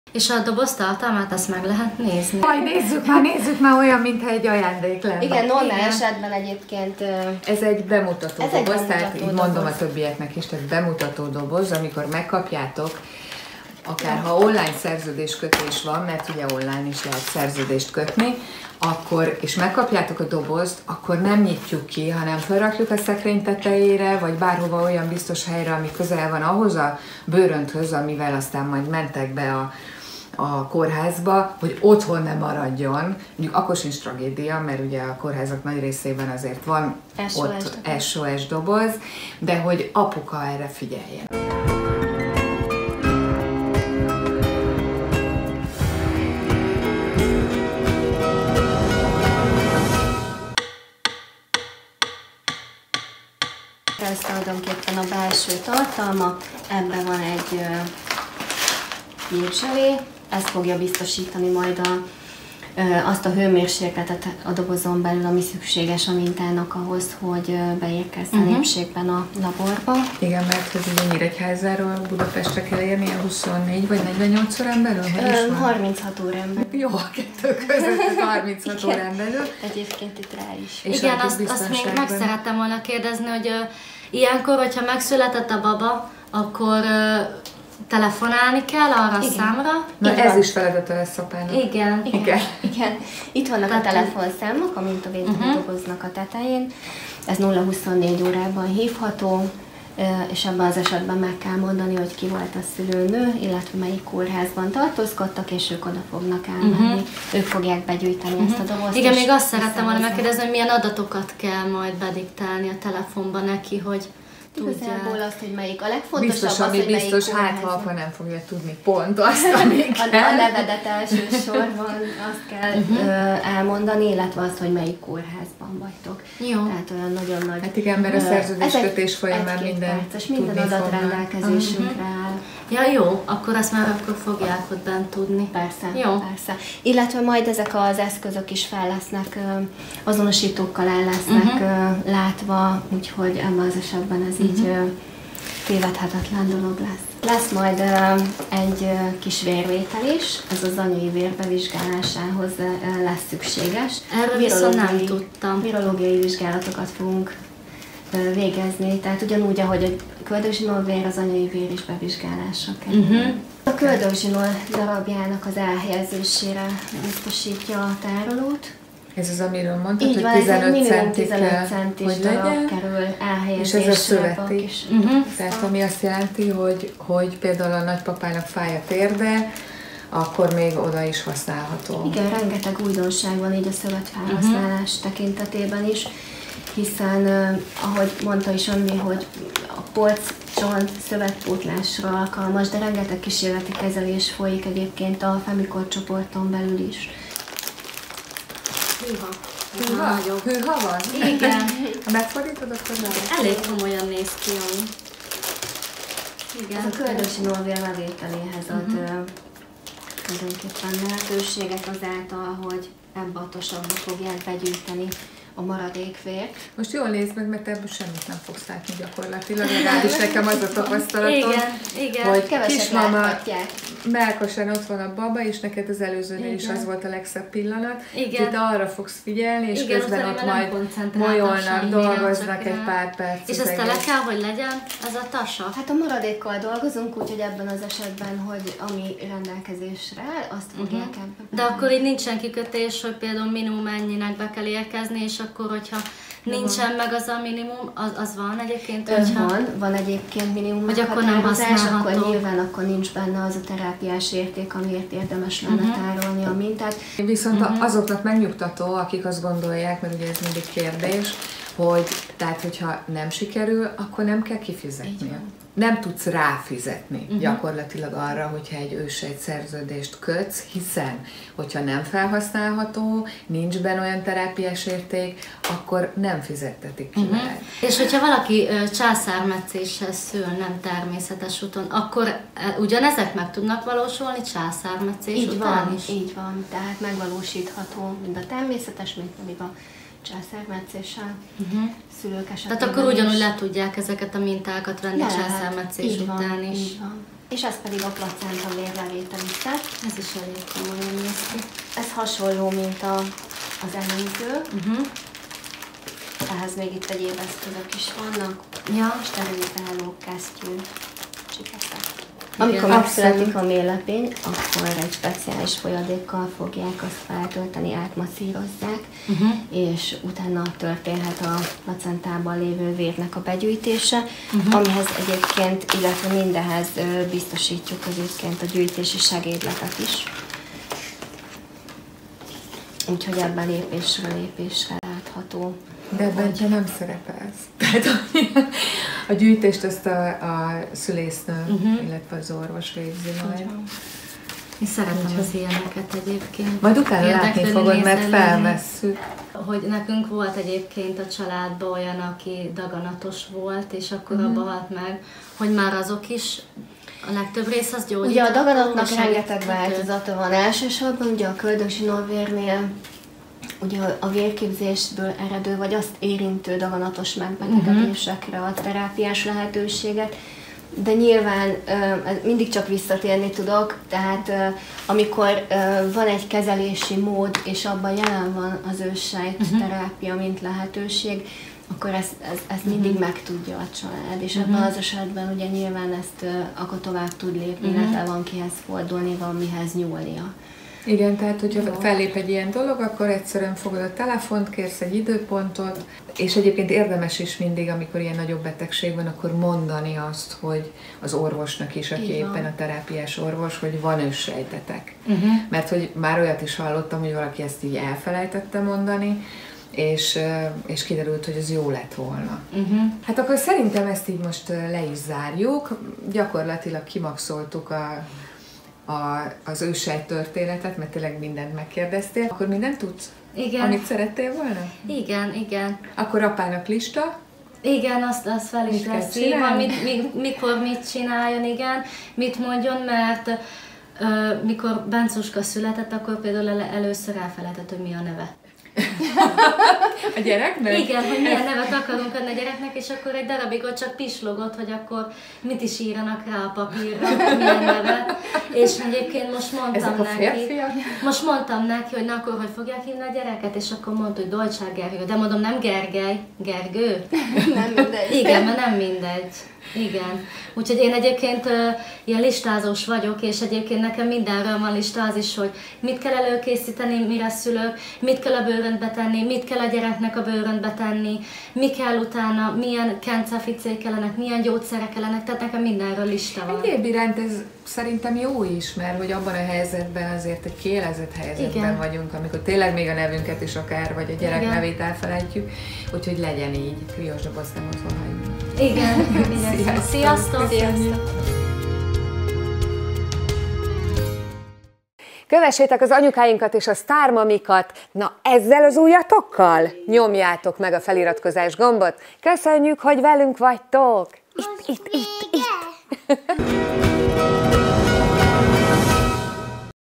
ParaMami férje a születéskor elfelejtette a gyermeke nevét. 6. rész
ParaPapi csak pislogott, amikor a csecsemős szerette volna kitölteni az adminisztrációt.
Ezért Anya már jóval a szülés előtt megírja a listát.
Köszönjük a a KRIO Intézet Családi Őssejtbank támogatását, hogy a kismamáink történetét megmutathatjuk Nektek.
ParaMaminak mindenről van listája. Mit kell a bőröndbe tenni a szüléshez és a csecsemőnek. Azt is a férje lelkére köti, hogy Kriós őssejtes dobozt hol tartsa, nehogy otthon maradjon. És az új baba neve is nagybetűkkel, aláhúzva: NEM Gergely, hanem Gergő!

?sub_confirmation=1